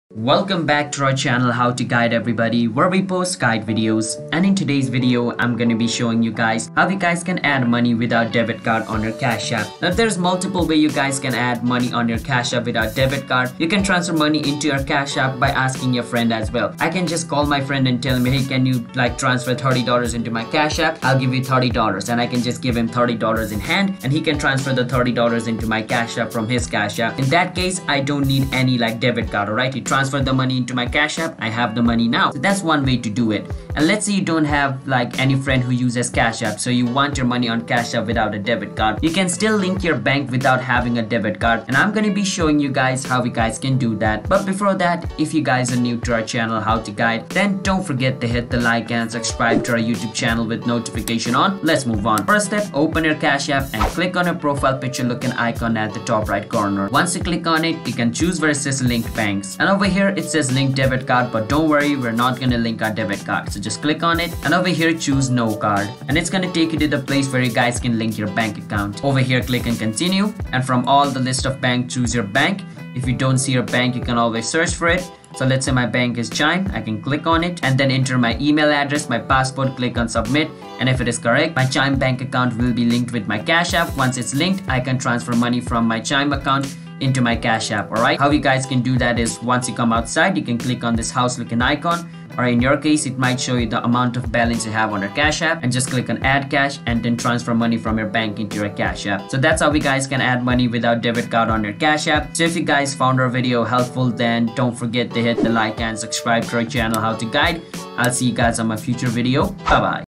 Cash App How to Add Money Without Debit Card (2023)
Learn how to add money without debit card on cashapp and how to add money on cash app.

Confused about how to add money to cash app without debit card? This video explains the exact steps on how to add money without debit card on Cashapp. Make sure you watch the video till the end to learn it in more detail. You can also transfer money into your app by asking a friend. But we're not going to link our debit card, so just click on it and over here choose no card, and it'll take you to the place where you can link your bank account. If you don't see your bank, you can always search for it.